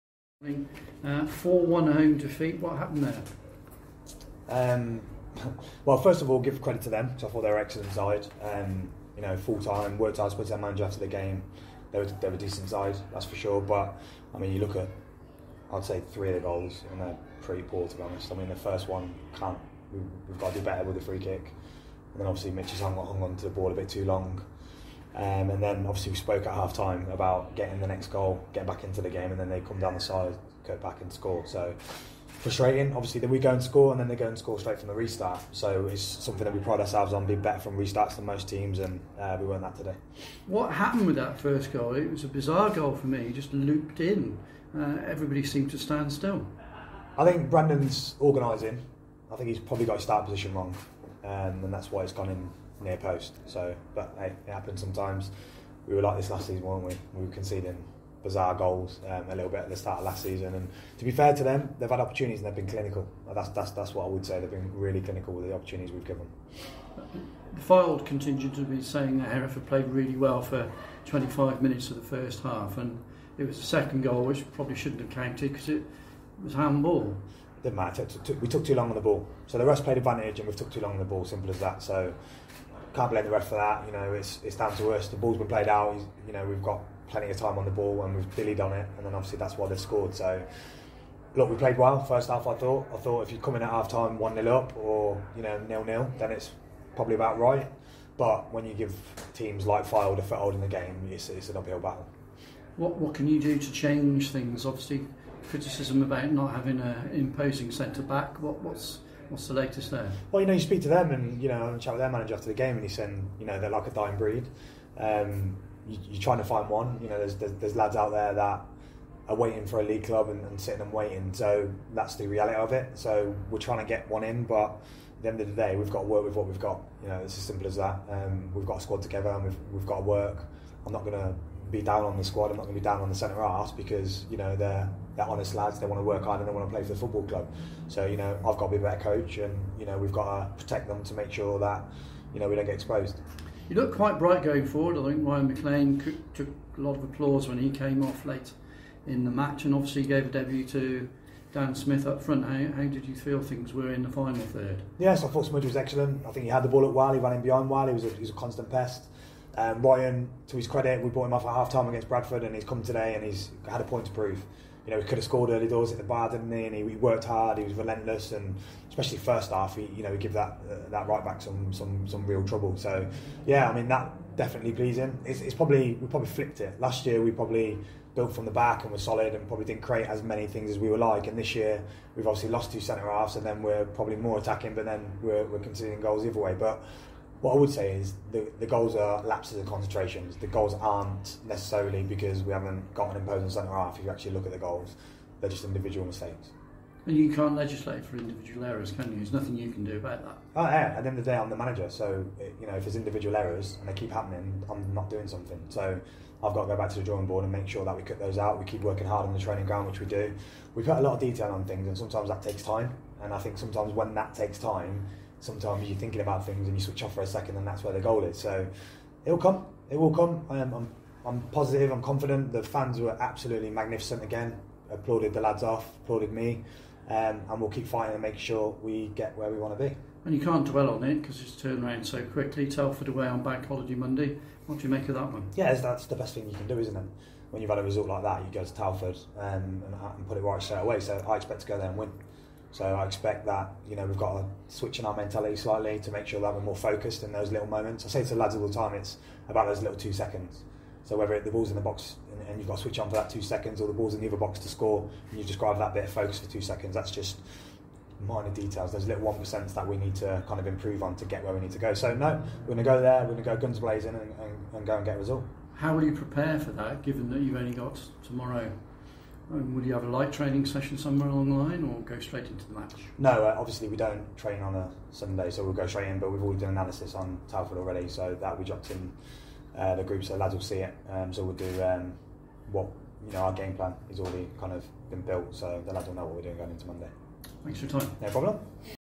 Gowling on 4-1 defeat to Fylde
Hereford manager Josh Gowling talks about his side's 4-1 defeat to AFC Fylde.